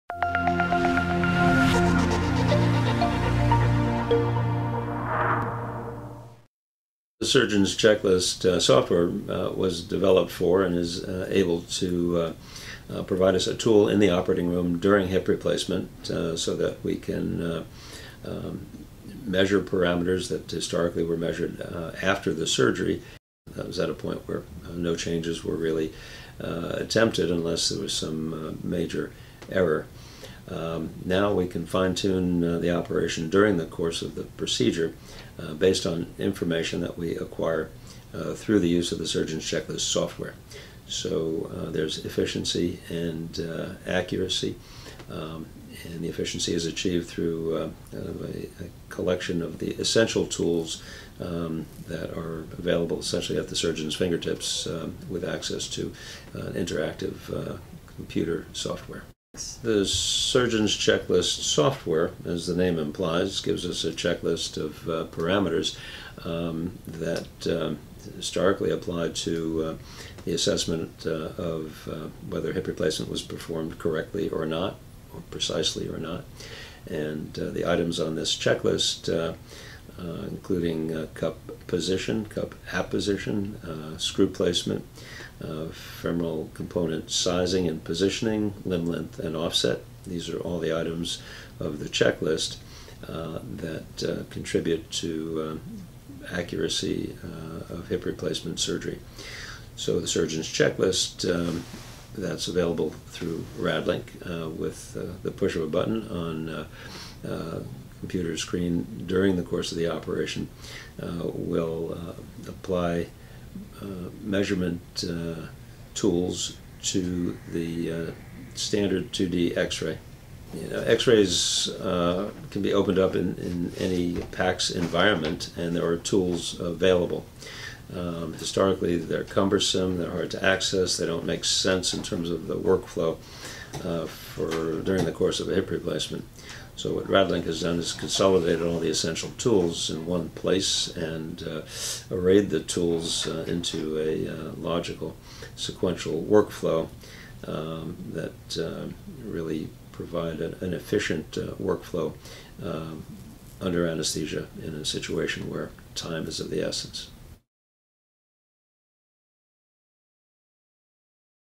Brad Penenberg, MD: Total Hip Replacement with Radlink
Brad Penenberg, MD, Division Chief, Hip and Knee Arthroplasty at Cedars Sinai Medical Center discusses his experiences using Radlink Surgeon's Checklist technology for Total Hip Replacement surgery.